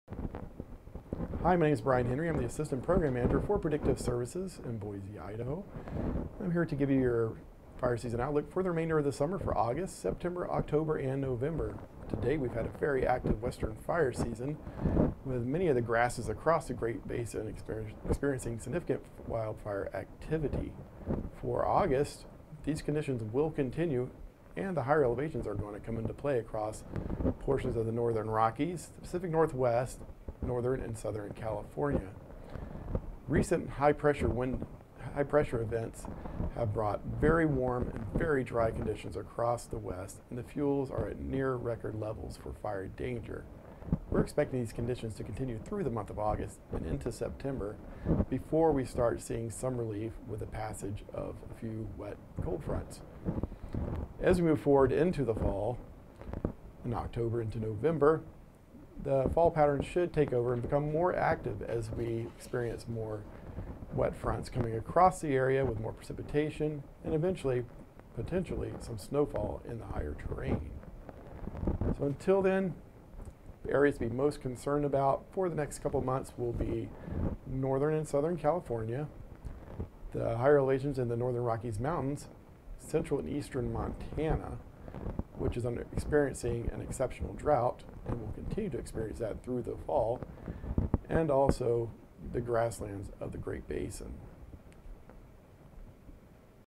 National Significant Wildland Fire Outlook - August 2017
Predictive Services releases its monthly fire season weather outlook for the months of August, September, October and November.